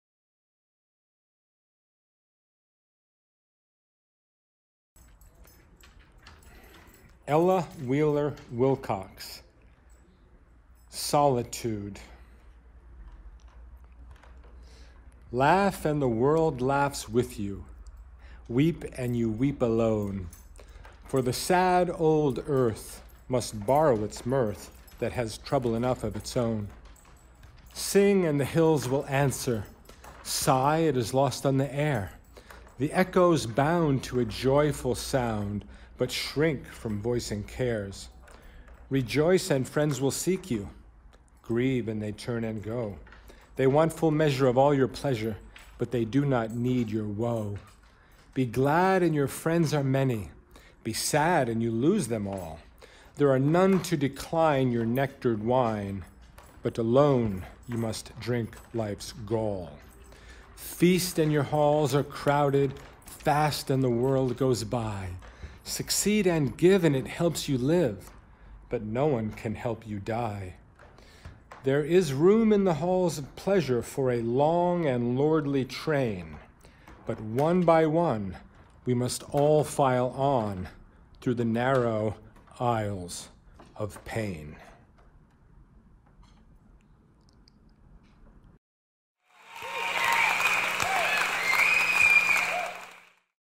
Ella Wheeler Wilcox, Solitude, recitation by Brian Paul Allison
From my personal anthology project, and the Art of Collecting Poetry. (Create your own Anthology. Stimulate your mind. Enrich your life.) Poetry Theater--tour de force with the Bard of the B---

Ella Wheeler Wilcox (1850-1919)
Solitude
Laugh and the world laughs with you,
Weep and you weep alone.
For the sad old earth must borrow its mirth
That has trouble enough of its own;
Sing and the hills will answer,
Sigh it is lost on the air
The echoes bound to a joyful sound
But shrink from voicing care
Rejoice and men will seek you.
Grieve and they turn and go
They want full measure of all your pleasure
But they do not need your woe.
Be glad and your friends are many.
Be sad and you lose them all.
There are none to decline your nectared wine
But alone you must drink life’s gall.
Feast and your halls are crowded.
Fast, and the world goes by.
Succeed and give and it helps you live,
But no man can help you die.
There’s room in the halls of pleasure
For a long and lordly train,
But one by one we must all file on
Through the narrow isles of pain.